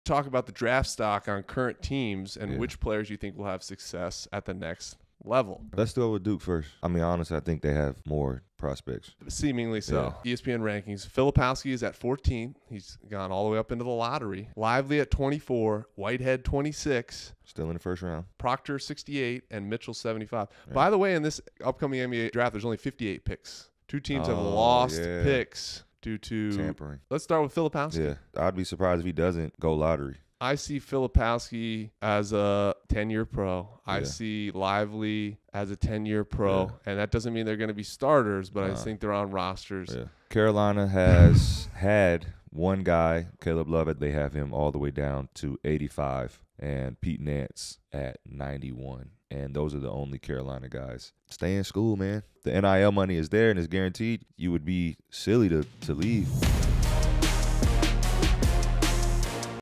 Why Duke & North Carolina Players Should NOT Declare for the NBA Draft This Year!
Creator, Co-Host & Executive Producer: James Michael McAdoo
Creator, Co-Host & Executive Producer: Ryan Kelly
Executive Producer & Co-Created By: @Devin Dismang
Produced, Directed & Designed by Assemble, LCC